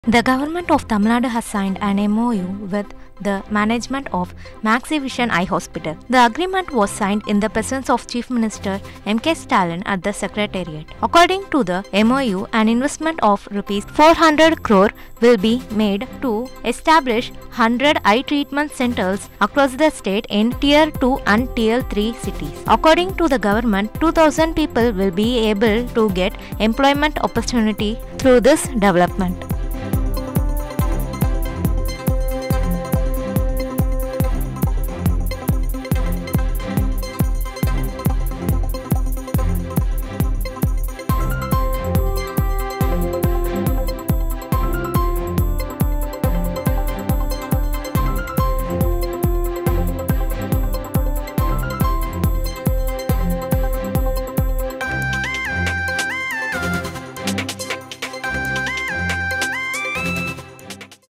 TN govt signed an MOU with Maxivision Eye Hospital | DT Next
The government of Tamil Nadu has signed an MoU with the management of Maxivision Eye Hospital. The agreement was signed in the presence of Chief Minister Stalin at the Secretariat. According to the Mou an investment of Rs. 400 crore will be made to establish about 100 eye treatment centres opened in Tier 2 and Tier 3 cities. According to the government, 2,000 people will be provided employment through this agreement.